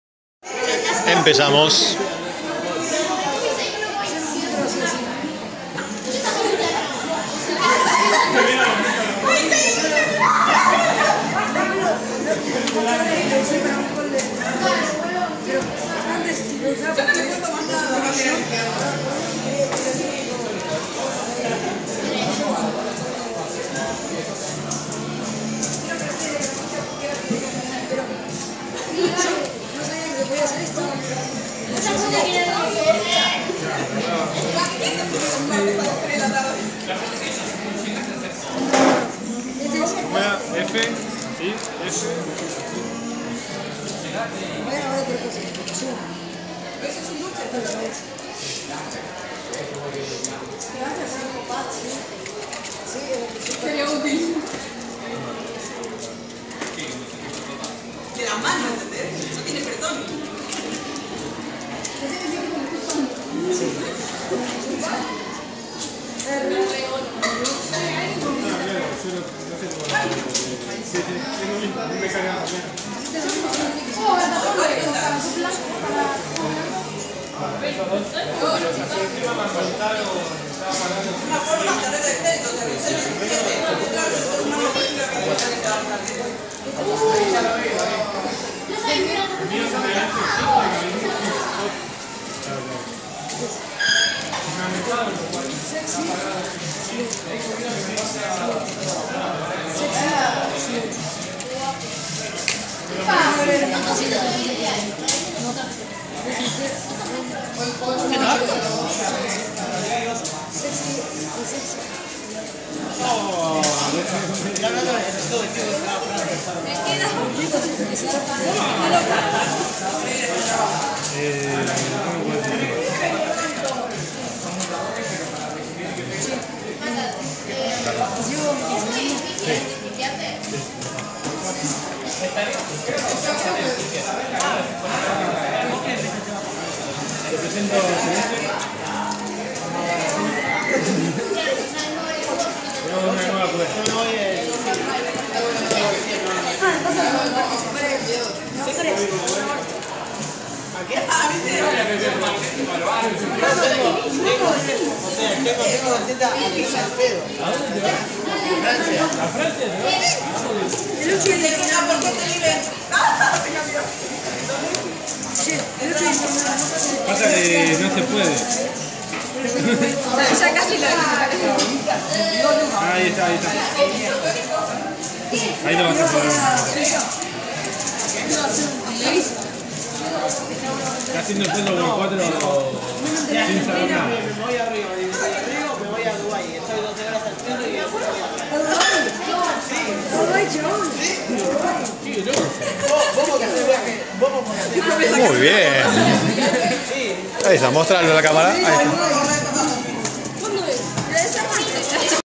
Isabella Rossi (7 años) resolviendo un Cubo 3x3
Reunión de Cuberos - Alto Palermo 13 de mayo 2017.